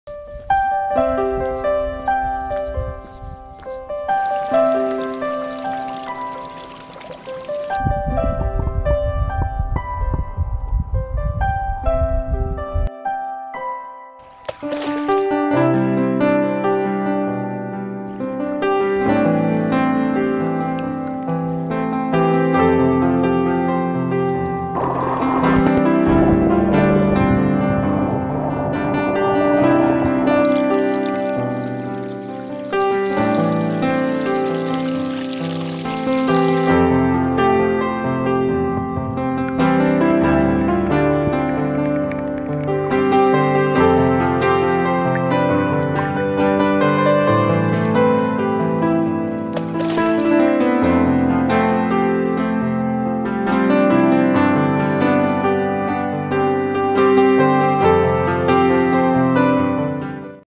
Acque
Credo che ognuno di noi abbia un posto segreto, magico, dove è possibile immaginare tutto. Il mio è in una goccia d'acqua. Vi ci porto con questo video che ho appena realizzato.